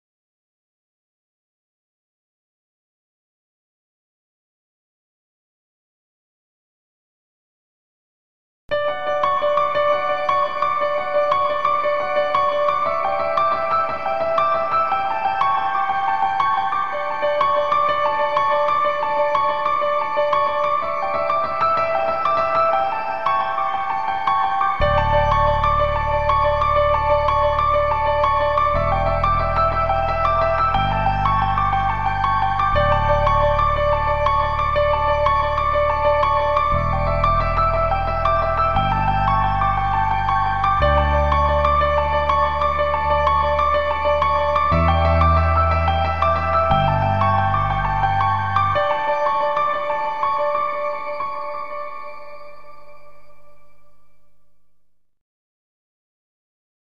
Strymon BlueSky With Synthesizer
Here's a little test with the awesome Strymon BlueSky.
It sounds really awesome and it just brings a whole new dimension to your sound!
I just used a simple piano sound in this vid.
BlueSky settings:
Decay 80%
Mode Shimmer
Low Damp 60%
Pre-Delay 55%
High Damp 40%
Mix 65%
Type Plate